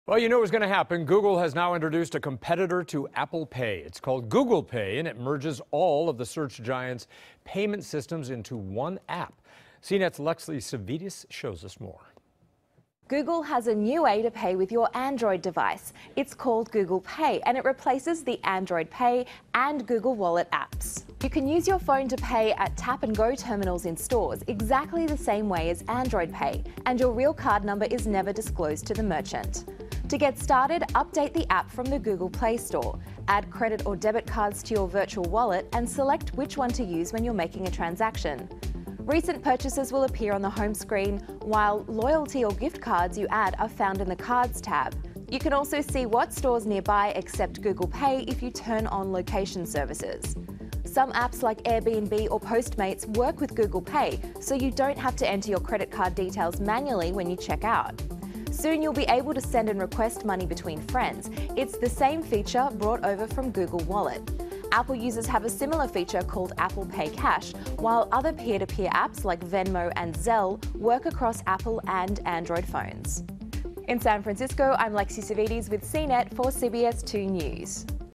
CNET Tech Minute: Here Comes Google Pay
The new service merges all of Google's payment systems in one app. Lexy Savvides reports.